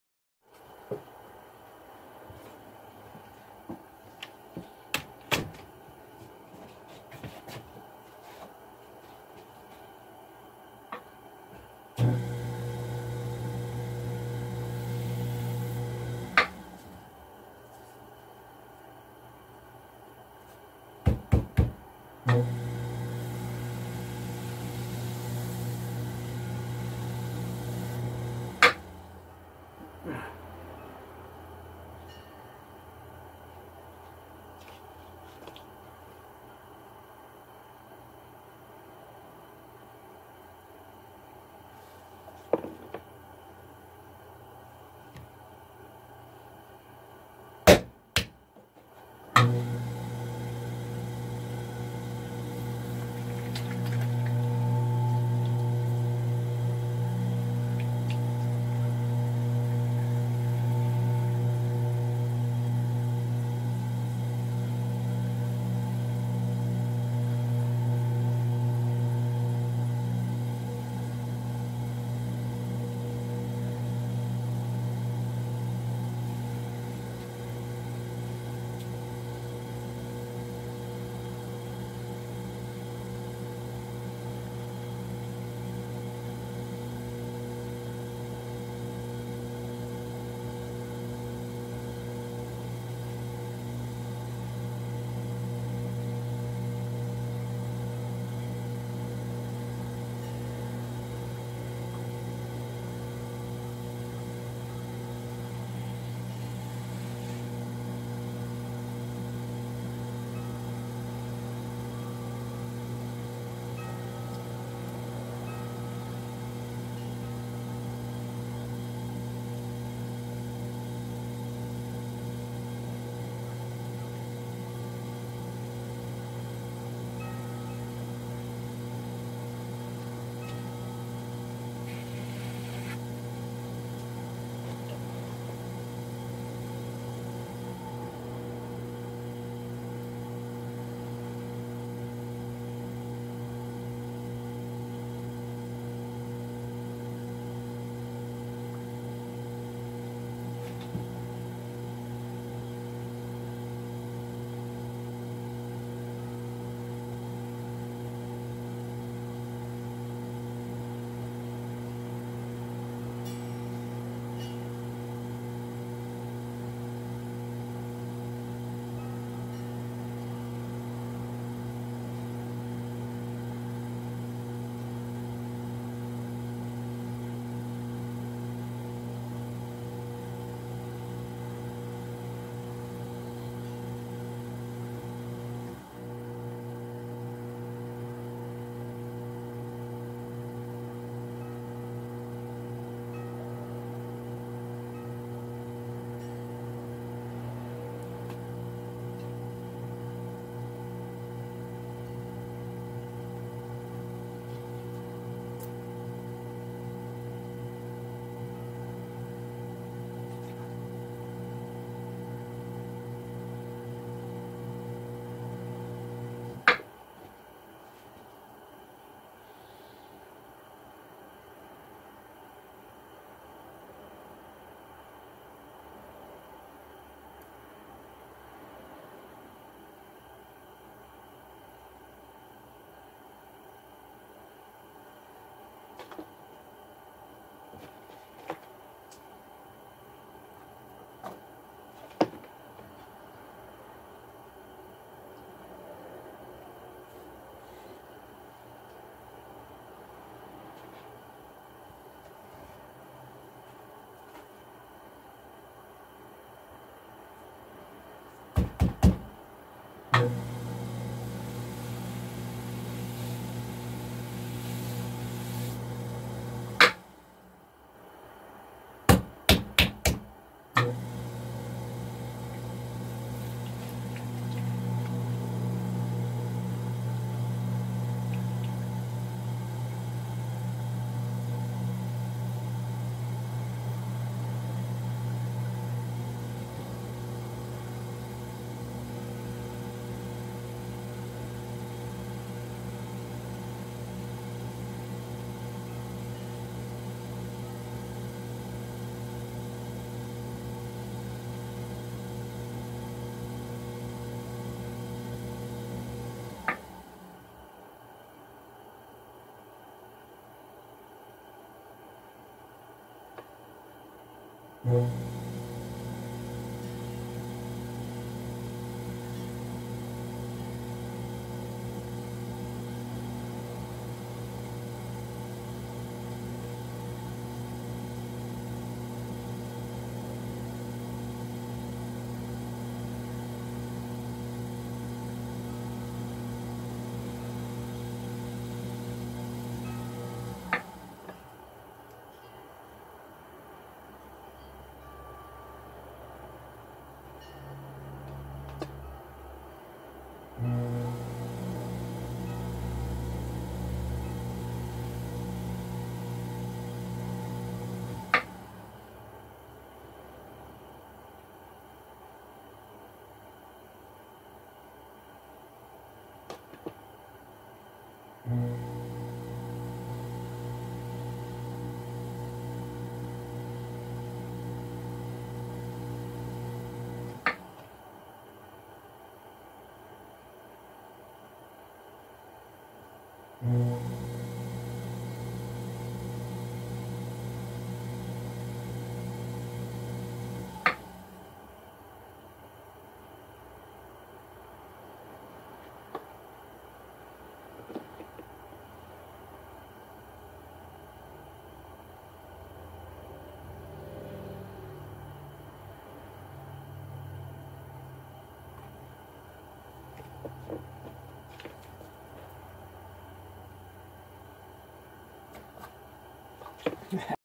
Another Honey Pot with Lid
Throwing a honey pot with lid on potter's wheel.